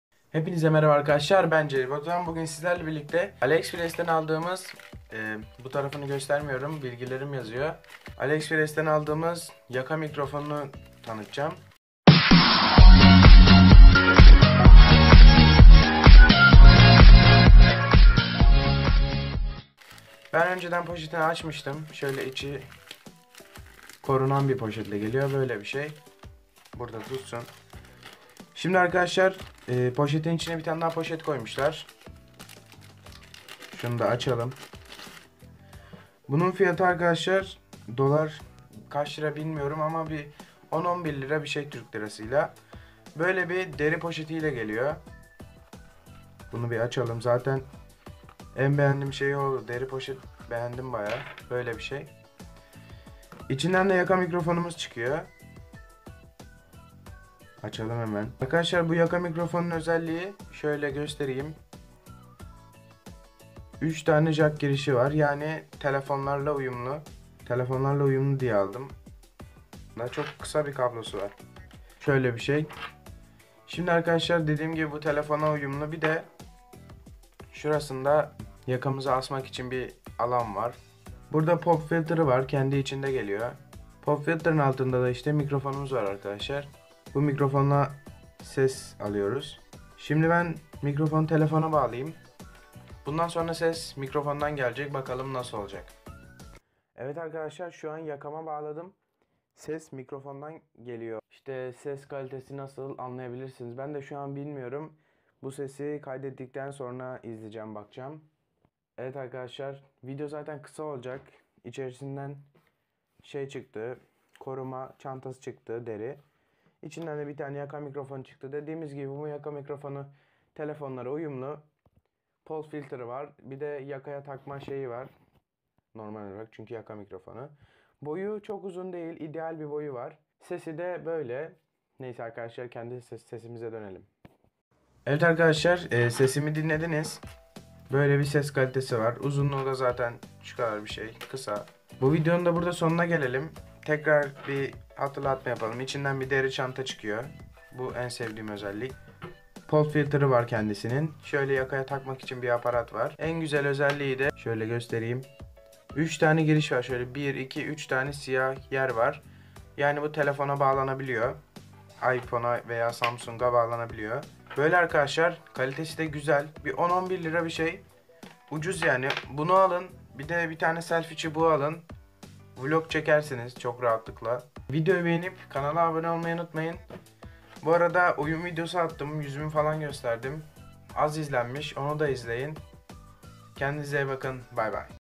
UCUZ VE KALİTELI YAKA MİKRAFONU KUTU AÇILIMI/İNCELEME!/ Aliekpress Alışverişim #2
Hepinize Tekrardan Merhaba Arkadaşlar Uzun Süredir Kutu Açılımı Yapmıyodum Yapayim d
Dedim Bu Videoda Çinden Aldığım Yaka Mikarafonunu Inceledim.